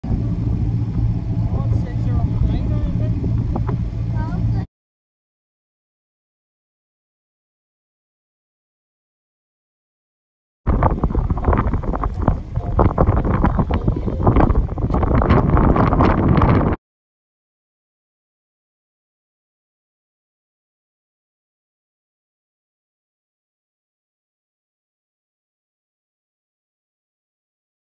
Boston Harbor Light Houses - Grave’s Light & Boston Light
Two great light houses in Boston harbor. Can only see by boat. Two gems that all should see in person.